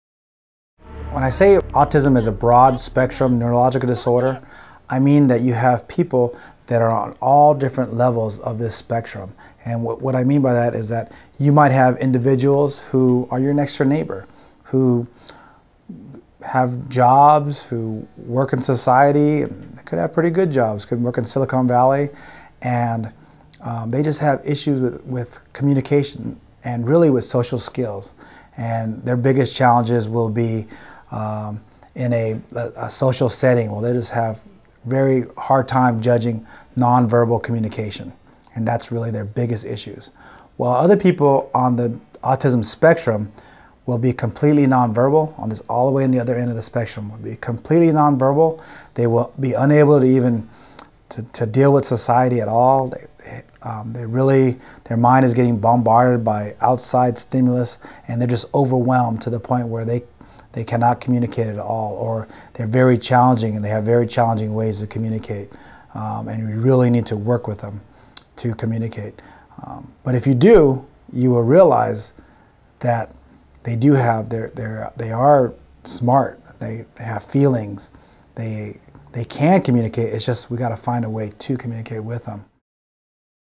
24-7 EMS: Pediatric Behavioral Emergencies: Autism ASD (Preview)
One of every 150 children born today will have some form of autism. It's important that all EMS responders are familiar with neurological emergencies, understand the complexity of these illnesses, and can recognize the signs and symptoms of ASD and PDD. Assessment and treatment techniques need to be modified to deliver successful and appropriate care. Featuring Ralph Carrasquillo, Firefighter/paramedic, San Diego Fire